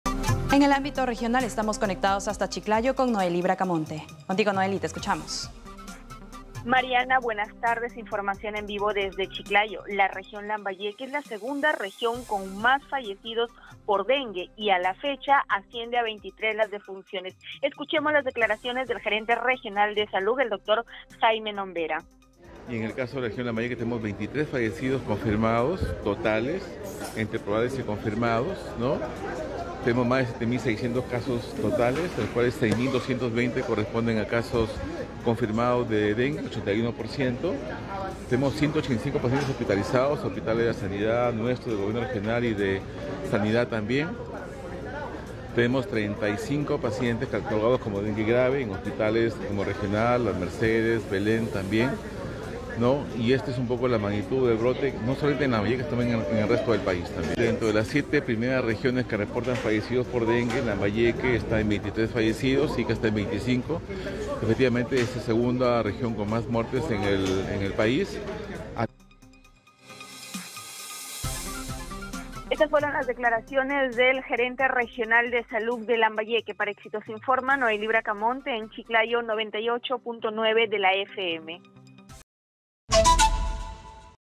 🔴🔵Chiclayo: Lambayeque es la segunda región con más fallecidos por dengue
Chiclayo: Lambayeque es la segunda región con más fallecidos por dengue y a la fecha asciende a 23 defunciones.

Exitosa Perú
"Exitosa: La voz que integra al Perú"

Corporación Universal